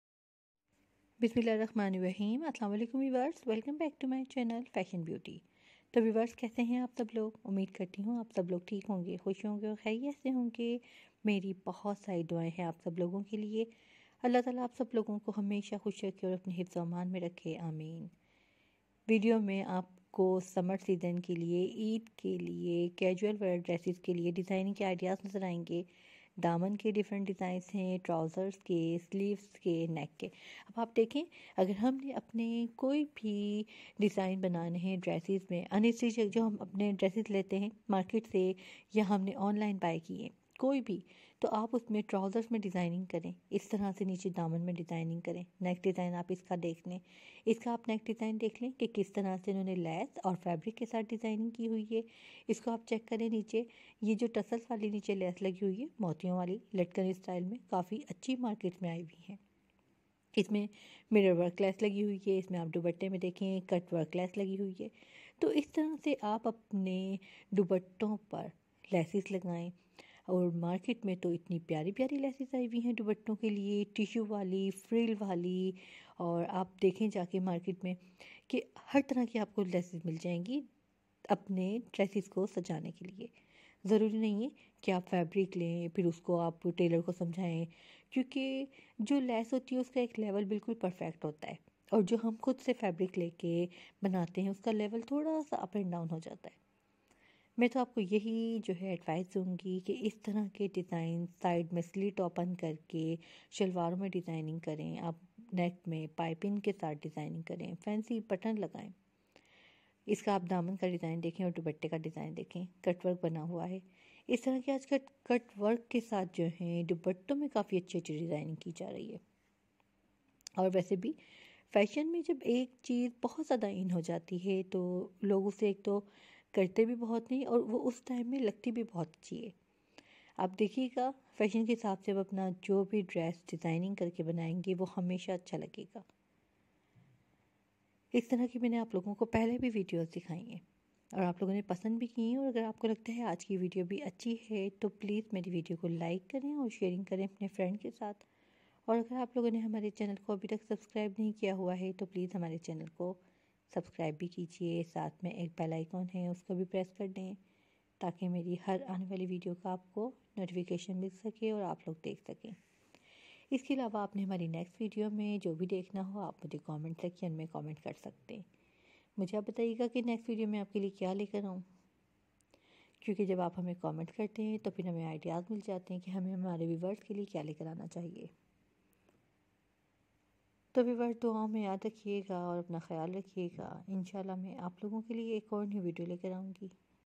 summer"Eid"Dresses DESIGNING Ideas & Outfits
Stylish Sleeves Designs Collection...
Hello freinds,,Today i will show you summer"Eid"Dresses DESIGNING Ideas & Outfits